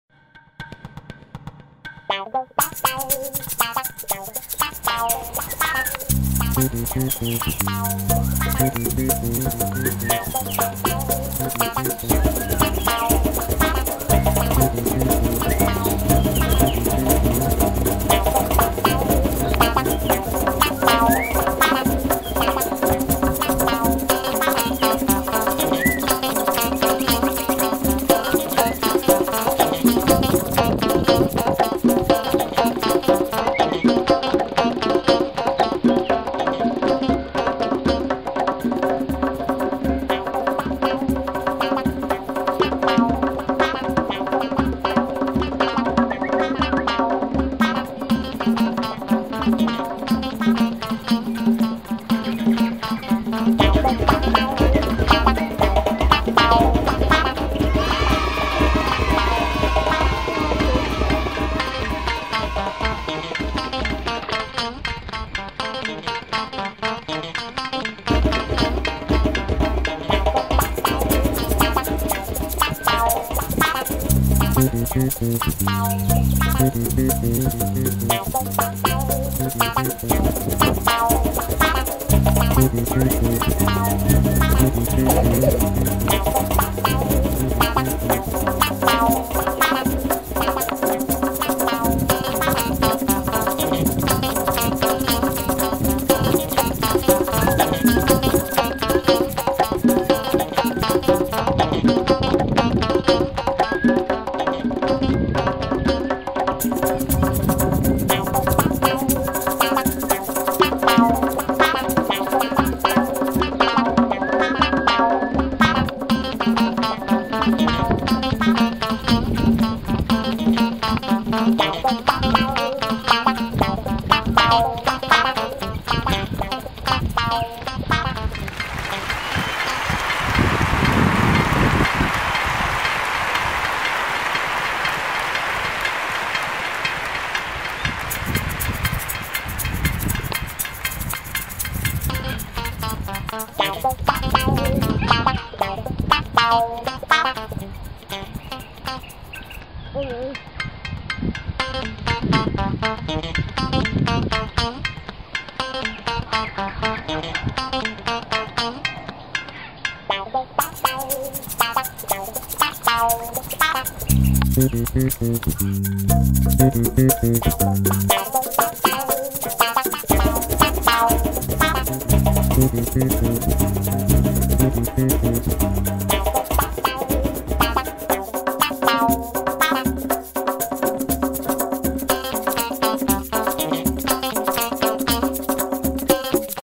Suffolk Park Postcard
A postcard of Suffolk Park's Tea Tree Lake (Sacred Lake to Aboriginal Ladies).
New South Wales, Australia.